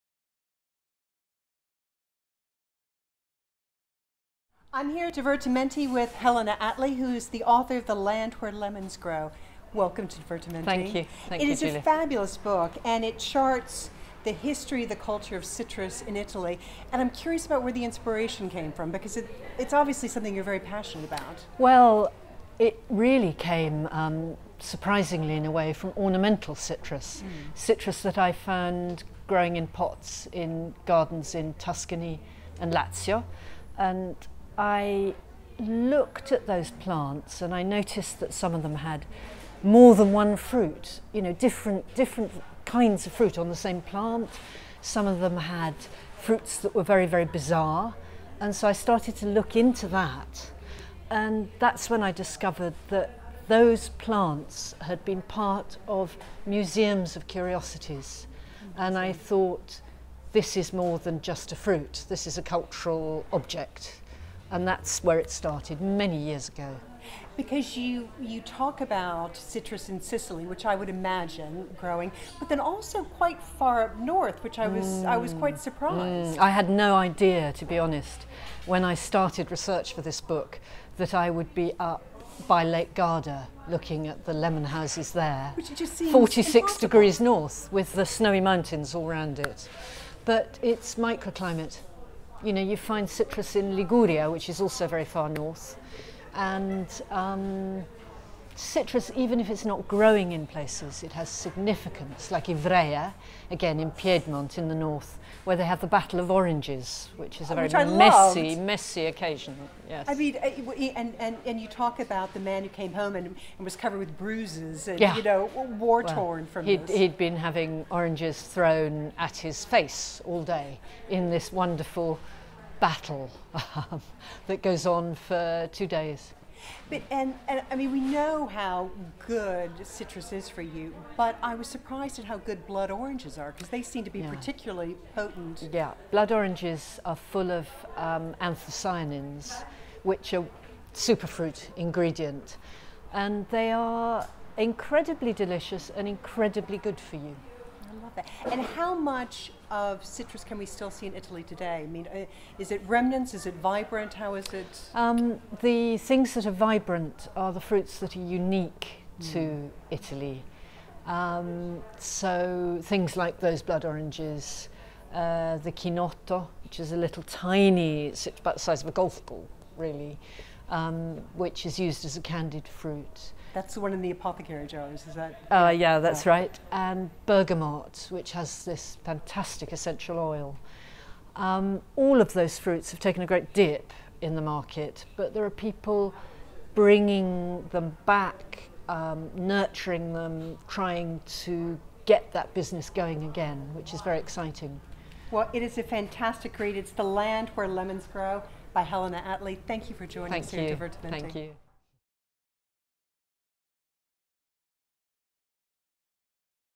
A chat with Helen Attlee, author of "The land where the lemons grow".
Helen Attlee speaks about her book "The Land where the Lemons Grow" with Julia Leonard at Divertimenti Culinary Salon.